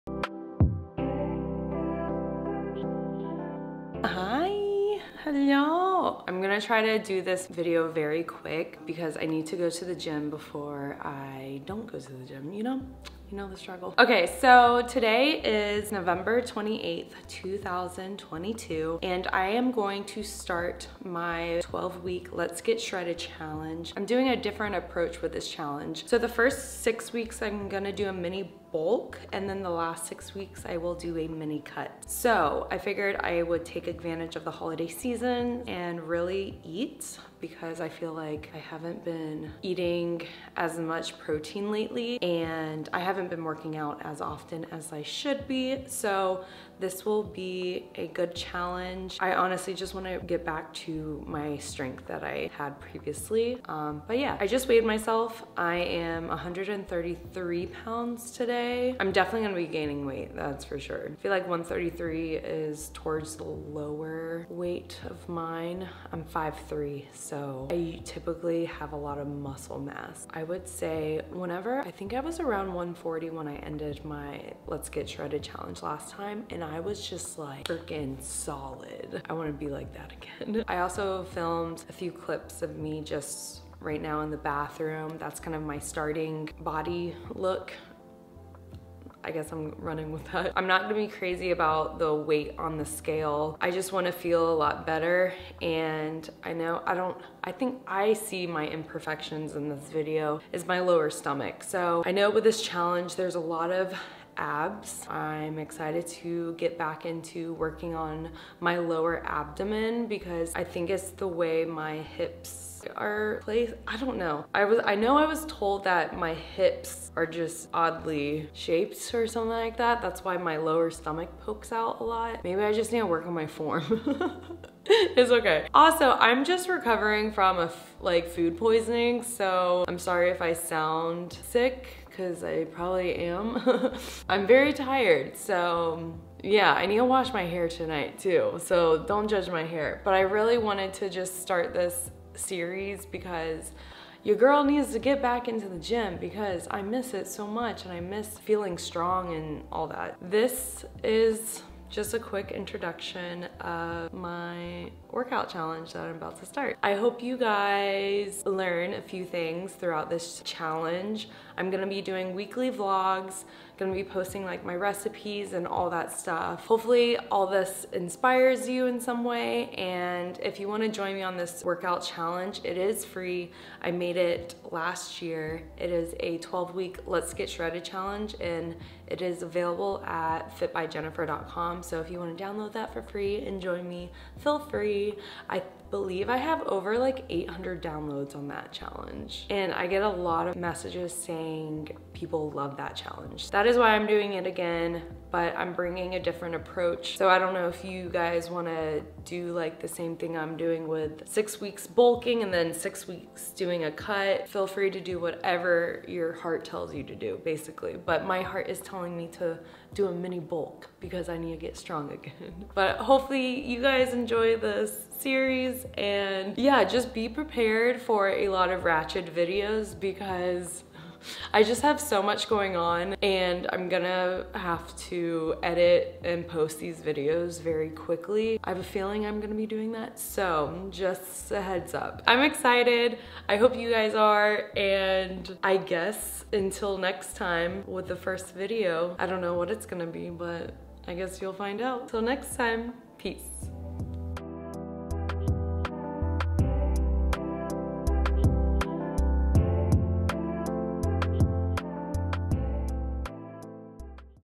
12 Week Bulk and Cut Workout Program
Welcome to my 12 week bulk and cut workout program series!

Join me on my free 12 week workout challenge and see the journey of my personal 6 week mini bulk and cut :D

If you're looking for a gym program that will help you get in shape, look no further! In this video, I'll be following along with a free 12-week gym program. This program is designed to help you lose weight, build muscle, and get healthy. So whether you're a beginner or an experienced exerciser, this program is perfect for you.